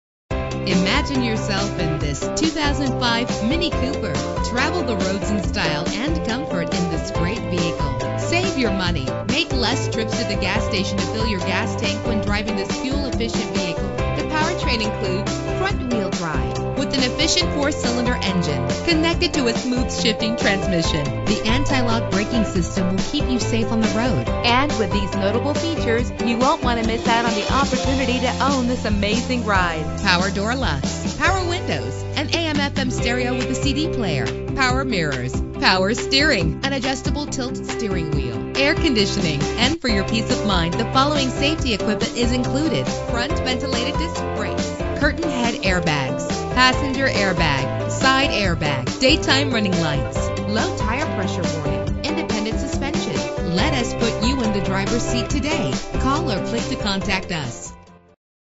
2005 MINI Cooper - Houston TX
Year: 2005
Make: MINI
Model: Cooper
Trim: Base
Engine: 1.6 liter inline 4 cylinder 16 valve
Transmission:
Color: British Racing Green Metallic
Mileage: 65842
Address:
17610 NORTHWEST FREEWAY